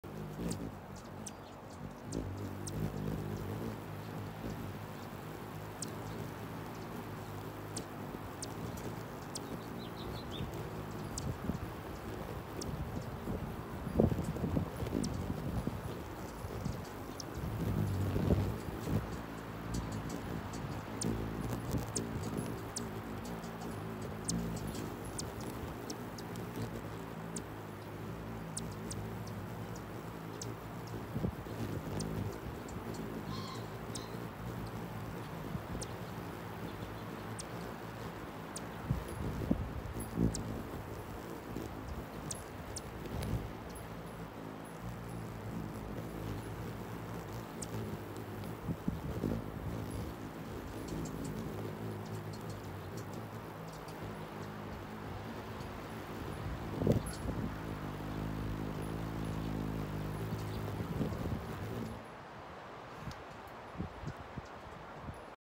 Three Hummingbirds Fight To Feed Out Of My Hand
Three ruby throated hummingbirds jockey for space at the feeder ring. Too cool!